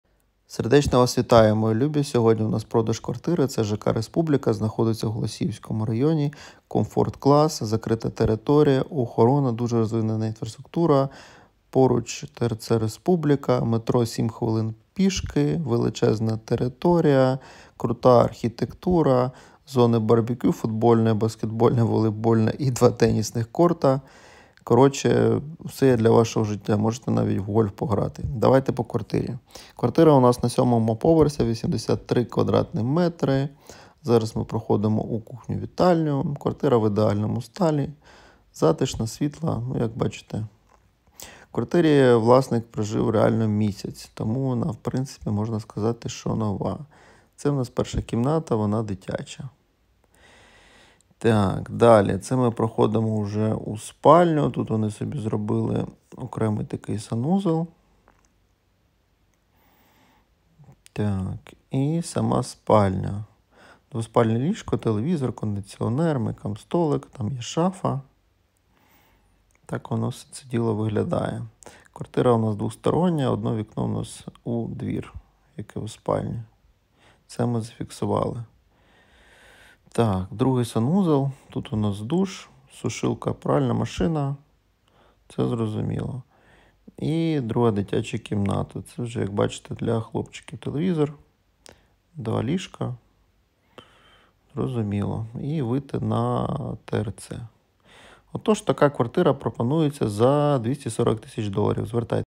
Продаж квартири ЖК Республіка Київ КАН Голосієво Теремки
Квартира з Ремонтом, меблями та технікою. ЖК Республіка. Один з найкомфортніших житлових комплексів Києва. Площа 83 м2 Поверх 7 з 9 вул. Рильського 1 Якісний ремонт, зроблений для себе по дизайн проекту. Квартира повністю готова до проживання нового власника! Поряд з ТРЦ Республіка. В пішій доступності метро Теремки! Трохи про сам комплекс: Територія міста закрита від сторонніх. Посередині знаходиться «Publik сentre» – зона відпочинку та громадський центр із унікальним ландшафтним дизайном. За американським зразком організується «Академія спорту», ​​в якій можна пограти у волейбол, баскетбол, футбол та Paddle-Tennis, тренуватися у TRX-зонах, користуватися душовими, вбиральнями, а за бажання безкоштовно взяти у прокат спортивний інвентар чи велосипед. У новобудові запроектовано: медичний комплекс; гімназія та дитячий садок; спортивні та дитячі майданчики, а також зони відпочинку та BBQ; тенісні корти, поле для гри у футбол, баскетбол, волейбол; мультиполі для дітей; місце для вигулу свійських тварин. З 7 чергою в комплексі розпочато новий етап будівництва, реалізація проекту продовжується з новим архітектурним стилем. Будується променад протяжністю близько 2,5 км з великою кількістю елементів інфраструктурного наповнення, таких як: амфітеатр; сухий фонтан; ставки; дитячі майданчики із водою; багатофункціональна площа щодо святкових активностей; коворкінг; велодоріжки. Більше квартир в тг каналі:prodazha_goloseevo